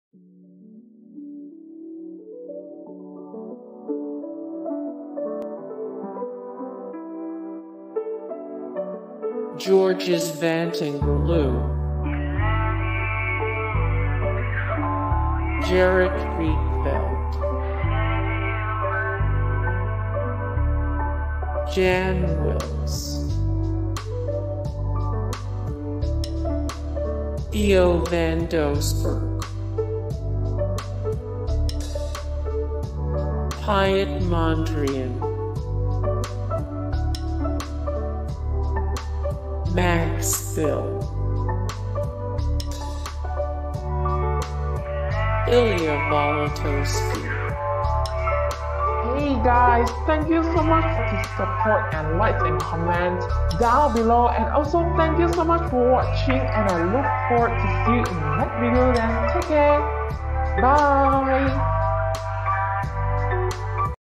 Famous De Stijl Artists 🤩
List of famous De Stijl artists, with images, bios, and information about their notable works. All the greatest artists associated with the De Stijl movement are included here, along with clickable names for more details on that particular painter or sculptor. These notable De Stijl period artists are organized alphabetically, but you may sort this information by clicking any column. Every well-known or influential figure in this movement is listed below. This list below has a variety of artists, like Anthony Kok and Max Bill, in it. {nodes}...more
0:00 - Intro
0:00:08 - Georges Vantongerloo
0:00:15 - Gerrit Rietveld
0:00:22 - Jan Wils
0:00:29 - Theo van Doesburg
0:00:36 - Piet Mondrian
0:00:43 - Max Bill
0:00:50 - Ilya Bolotowsky